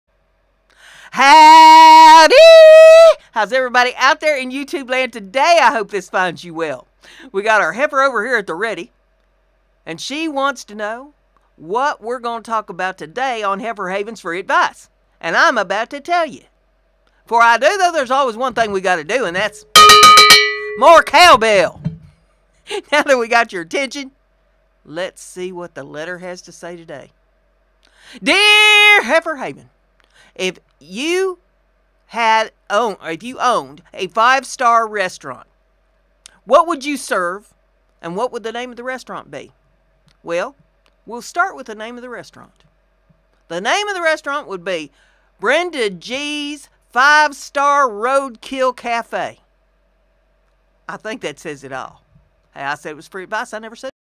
Heifer Haven Free Advice - What would you name your 5 star restaraunt?
This is a series of free advice from Heifer Haven.  I said FREE...not good!  Todays topic...Question today- What would you name your 5 star restaraunt and what would you serve?  Here at Heifer Haven I have the answer.  Here's FREE Advice...didn't say it was good.

My Etsy Store